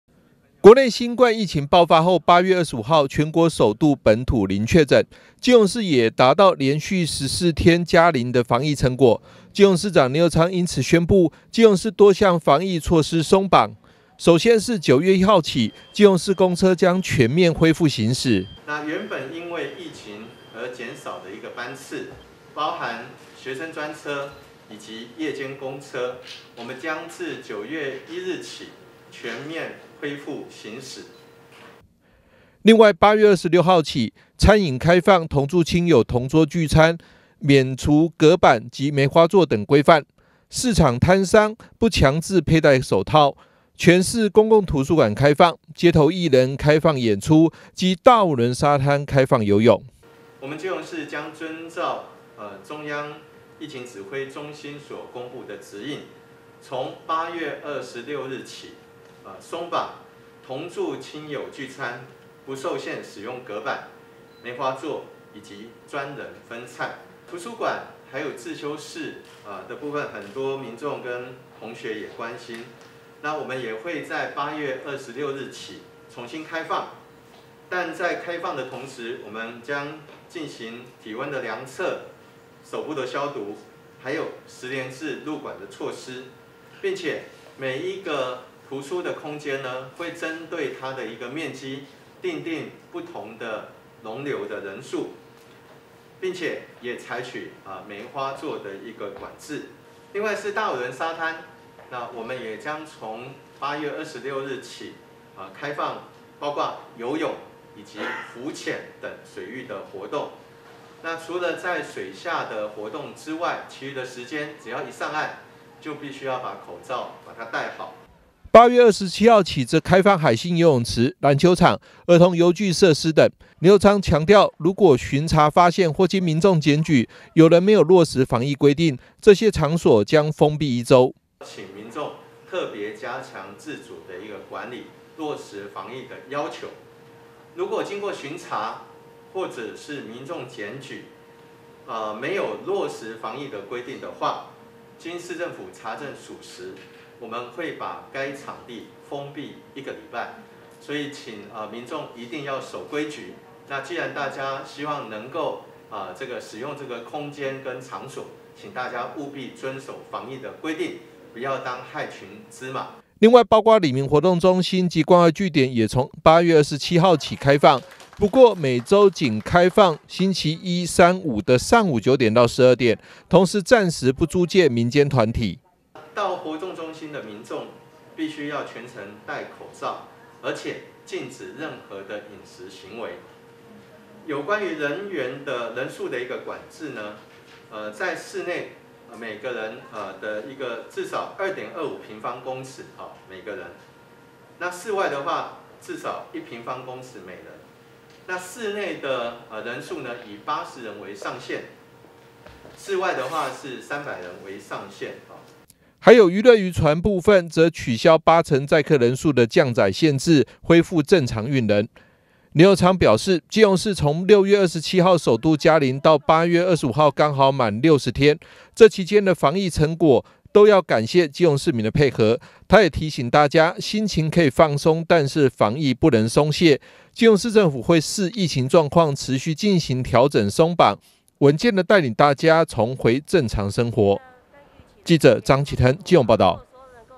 新聞 基隆|疫苗 施打|防疫 指引|開放 解封|恢復 正常生活|中嘉新聞 2021|基隆連續14天零確診 逐步放寬 林右昌宣布十多項管制措施鬆綁
基隆連續14天加零基隆市長林右昌宣布多項管制措施鬆綁如有違規將封閉一周

（中嘉吉隆新聞記者張啟騰/基隆報導）全國疫情進入第三級警戒後，8/25首度本土零確診，基隆也連續14天零確診，基隆市長林右昌因此宣布逐步鬆綁十多項防疫措施，包括餐飲內用規範、市場管制、圖書館及大武崙沙灘水域活動都從8/26起放寬相關措施，8/27起則開放海興游泳池籃球場兒童遊具與體健設施里民活動中心及關懷據點等，9/1起因為疫情而減少的基隆市公車班次，包括夜間公車及學生專車等也全面恢復正常行駛。